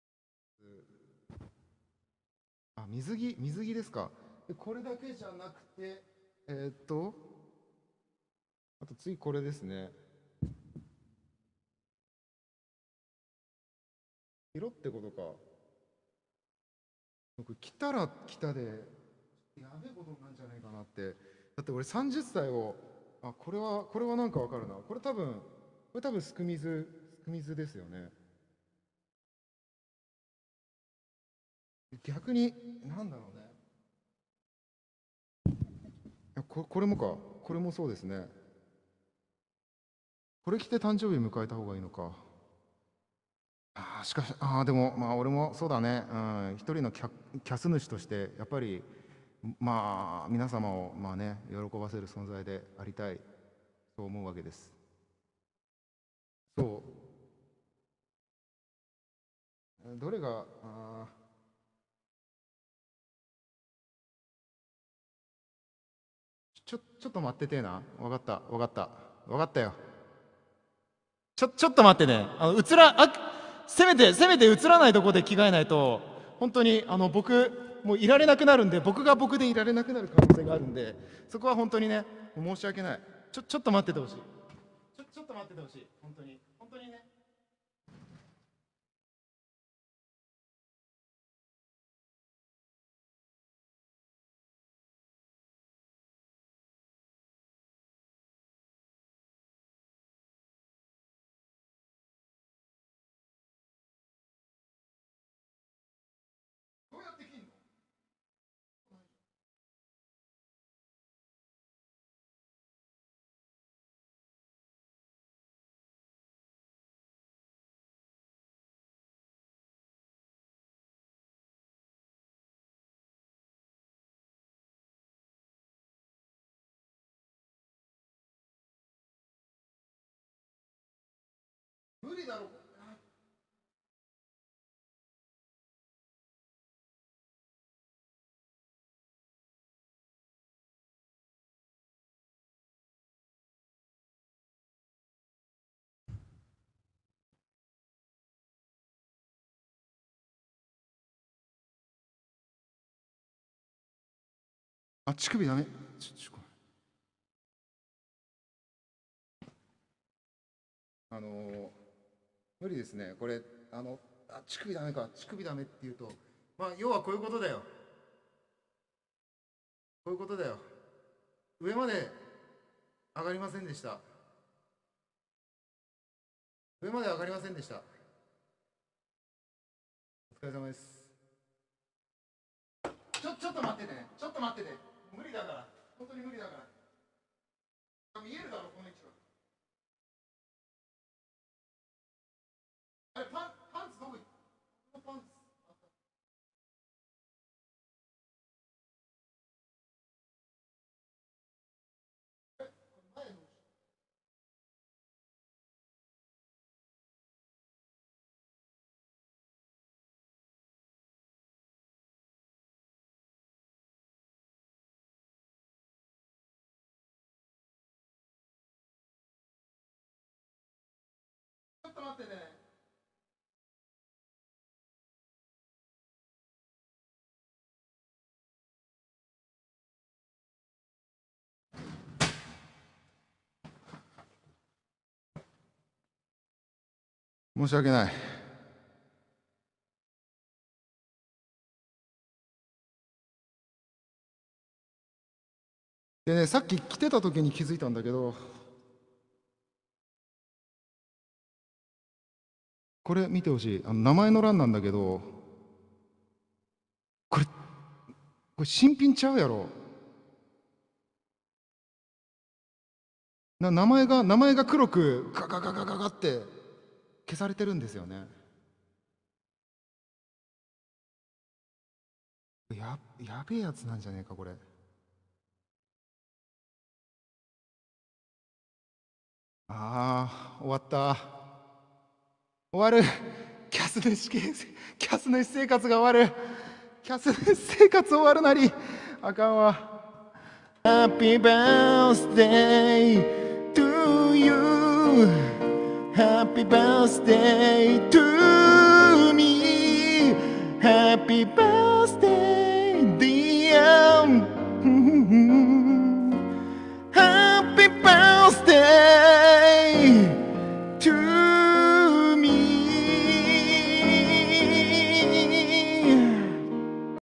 自宅に届いたスク水を着てみるゆゆうた兄貴
2018/7/1〜7/2のツイキャスより

ゆゆうた兄貴30歳のお誕生日おめでとうございます!!

誕生日迎える瞬間脱糞(弾き語り)してて草